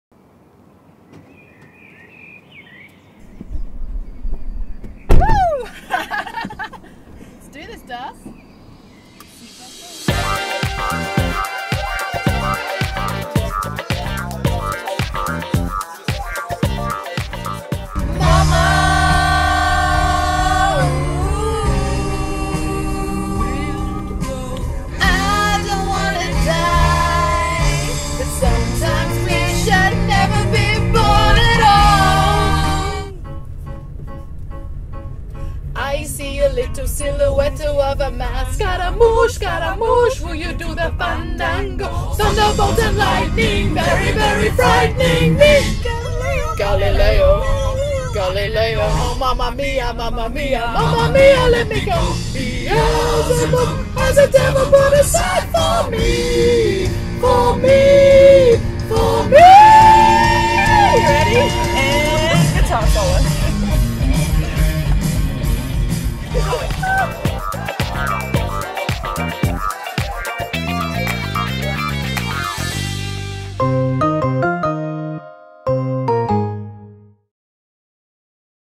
MCT Carpool Karaoke Ep.3
Carpool Karaoke continues this week thanks to Mike Carney Toyota.

Check out championship heroes Darcee Garbin and Rachael McCully belting out this Queen classic!